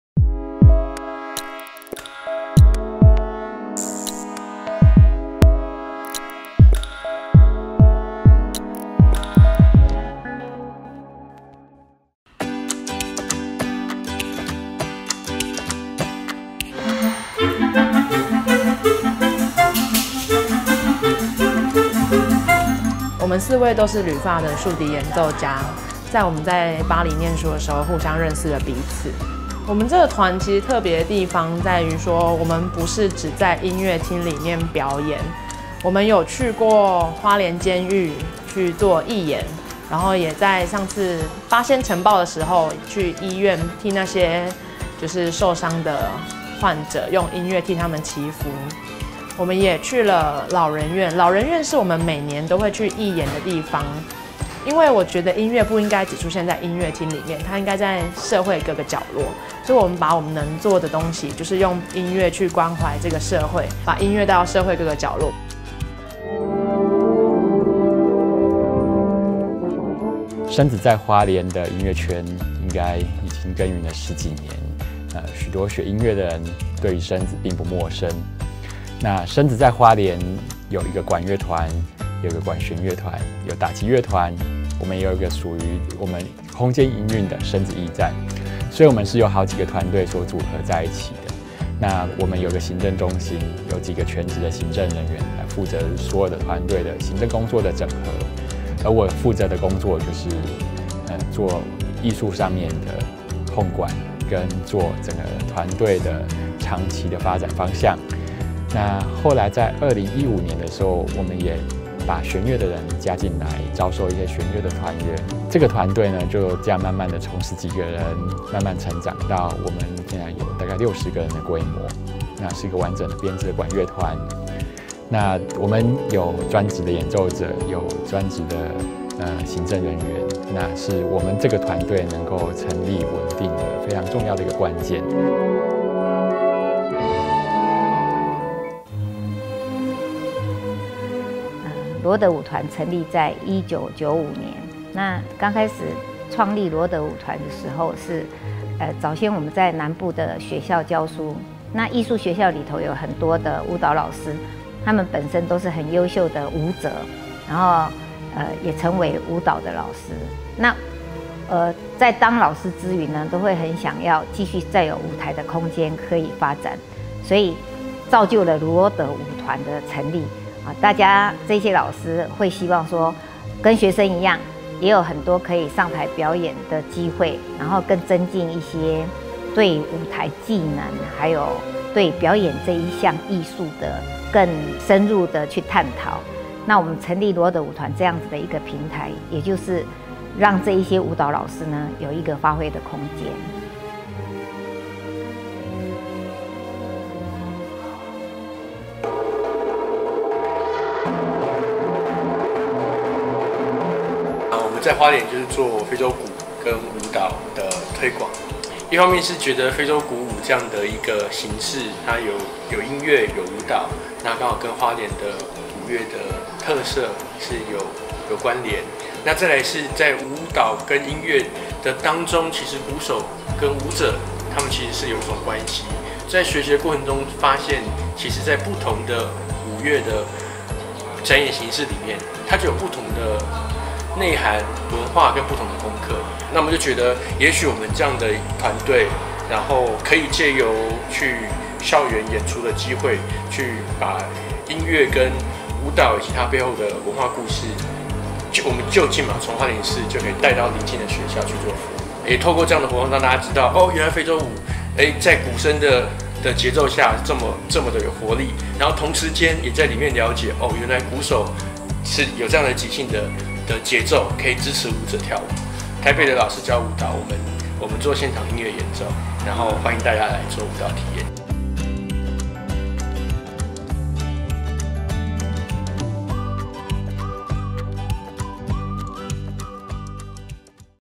｜２０１８花蓮傑出表演團隊｜簡介影片
今年度共有四組優秀的表演團體
獲得２０１８年花蓮傑出表演團隊的殊榮
分別是High Fly豎笛四重奏
聲子樂集
羅德芭蕾舞團
以及WASSA sogood鼓舞樂團

快來一起透過影片來了解這些優秀的表演團隊吧！